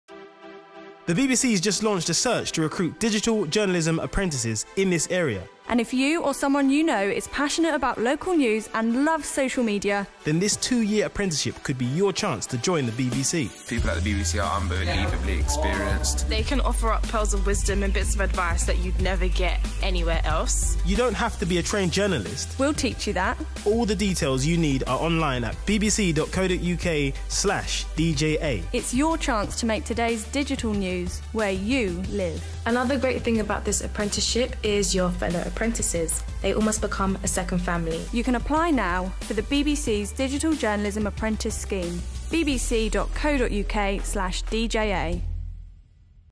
Digital Journalist Apprenticeship at BBC Three Counties
Do you love social media? Are you hardworking, flexible and creative?

Maybe you'd be interested in our Digital Journalist Apprenticeship at BBC Three Counties in Dunstable.